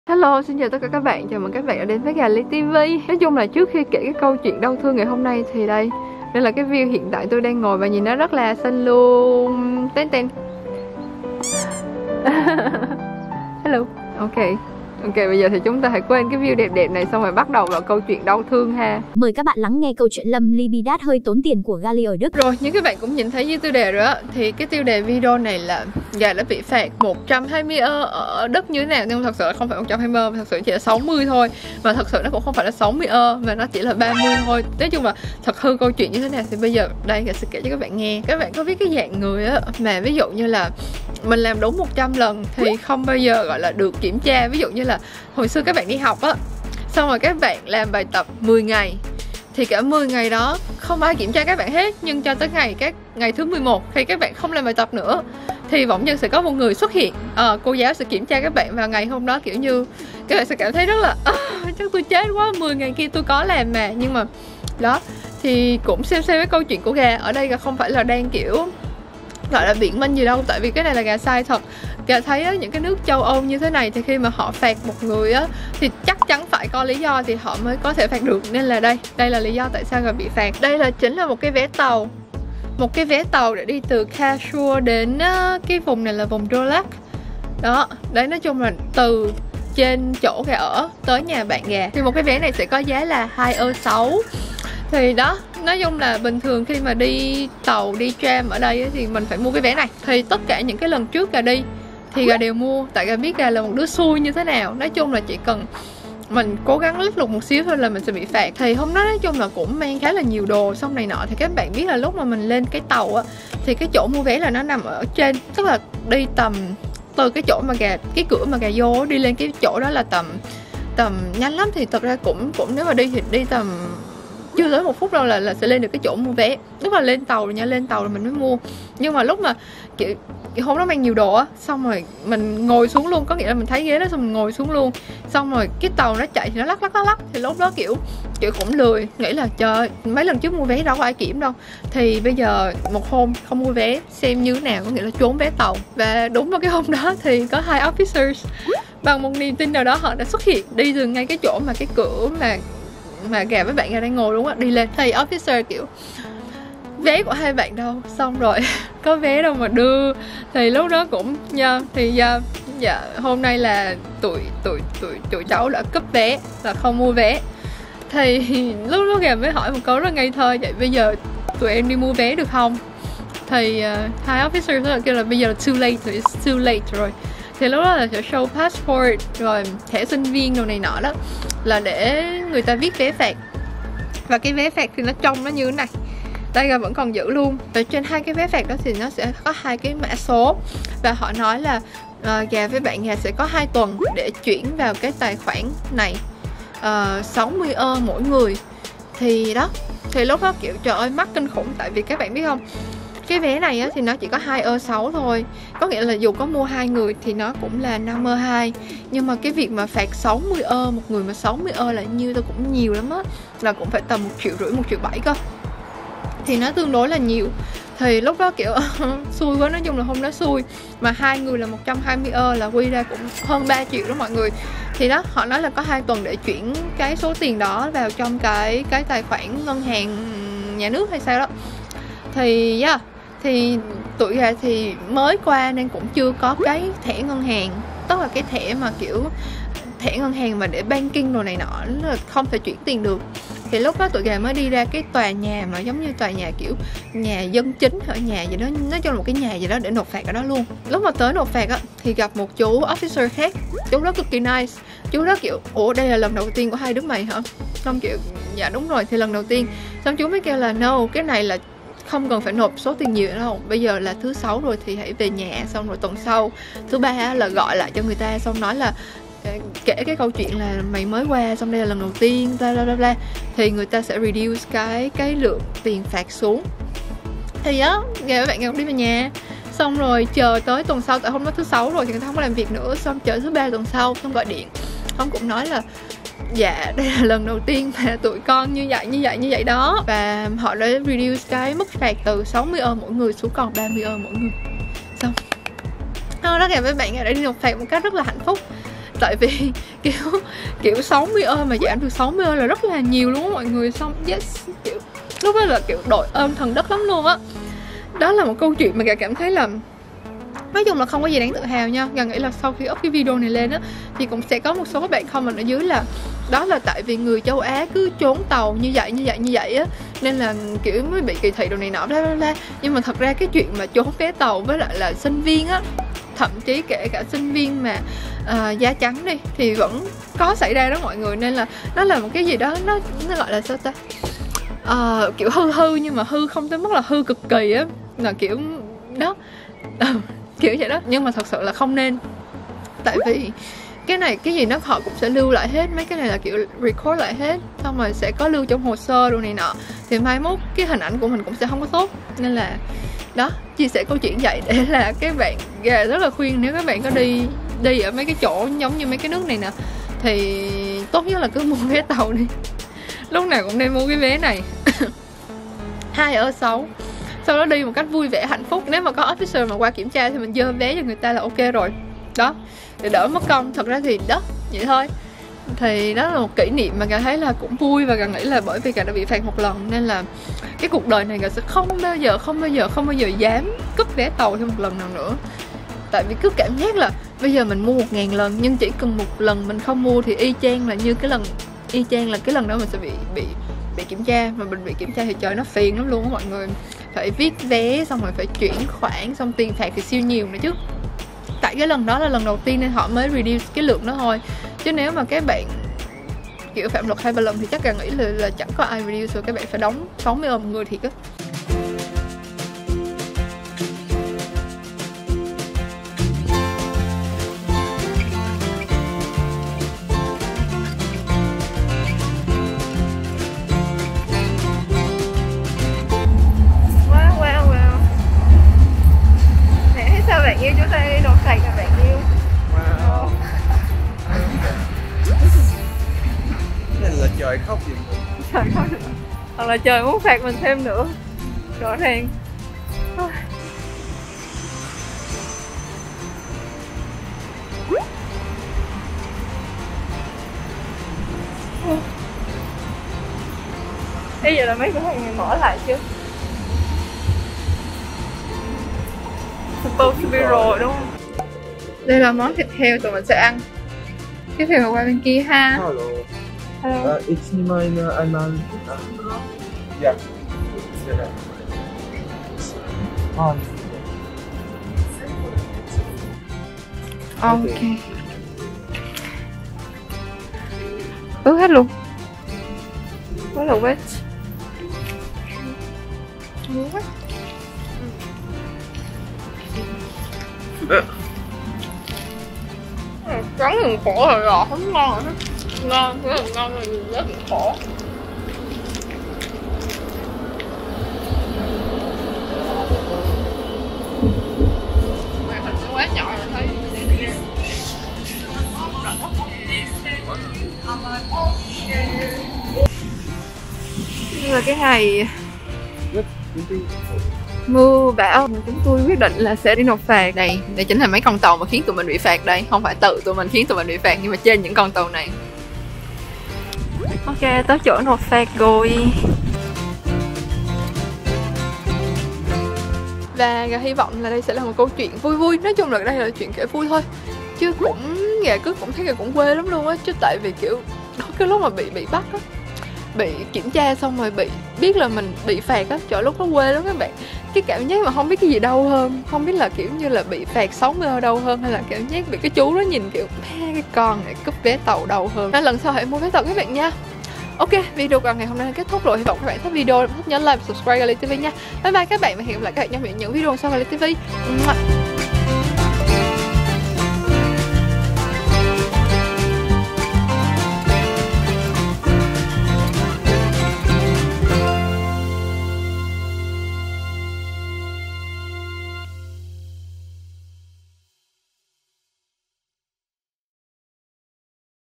Mình đã bị phạt €120 trong tuần đầu ở Đức như thế nào 🤭 Kể chuyện để đừng ai giống Gà nha
♥ Theo dõi Gali nha :
© Bản quyền thuộc về Gali TV
© Copyright by Gali TV ☞ Do not Reup